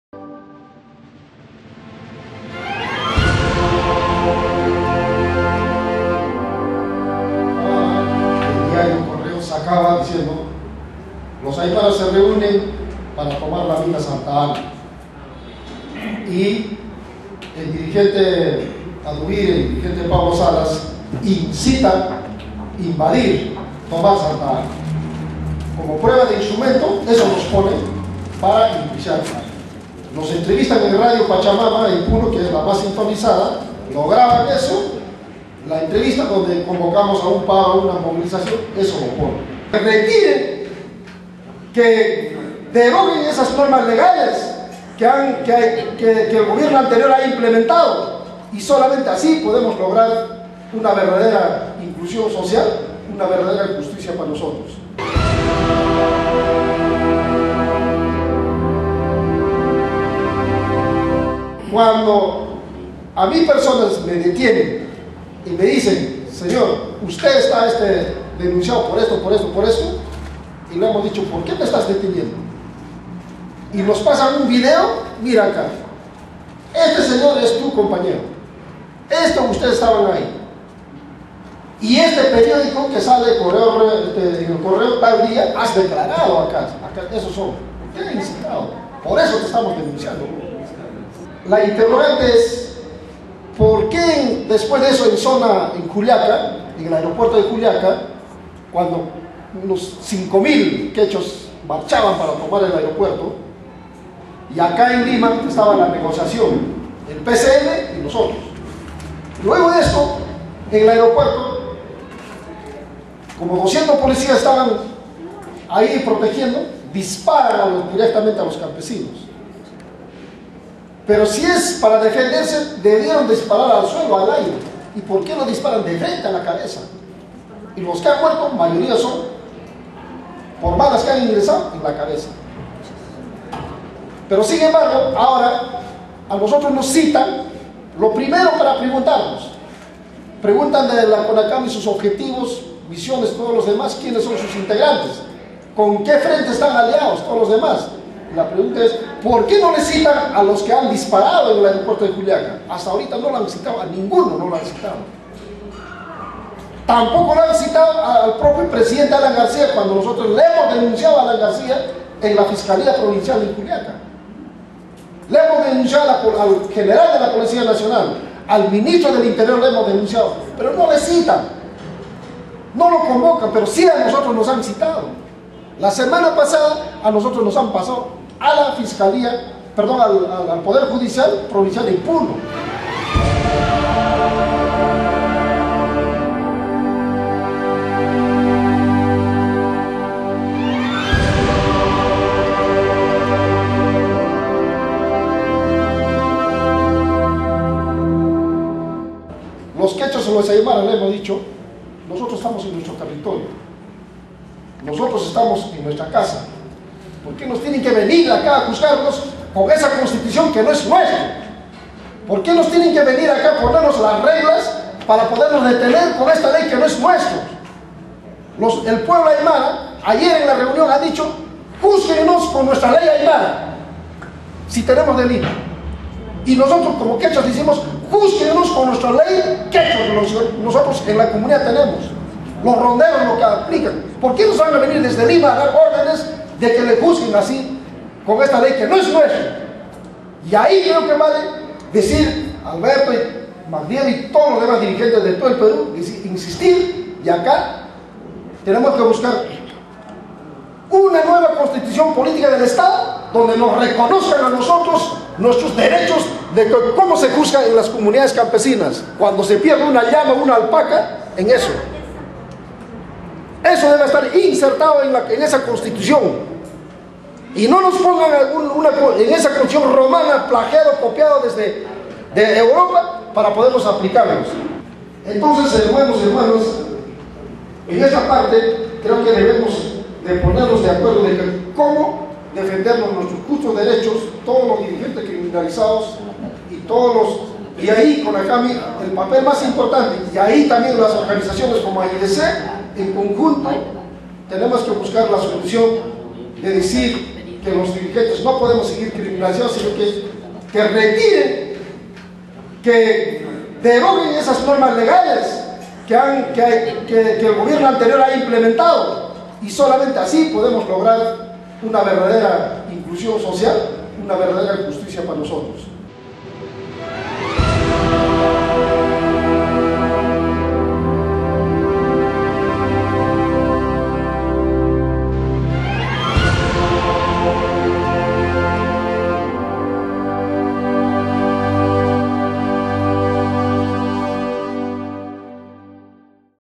Testimonio Pablo Salas
El Dirigente campesino quechua Pablo Salas Charca, enfrenta persecusión judicial por parte del Estado por defender el territorio de sus comunidades  ante la invasión de las industrias extractivas, la medida de fuerza ejercida por la region Puno en Peru, en contra de la instalación del proyecto minero Santa Ana, el pasado mayo de 2011, culminó con la muerte de 4 personas y cientos de heridos tras la  violenta represion policial sucitada en el aeropuerto de Juliaca.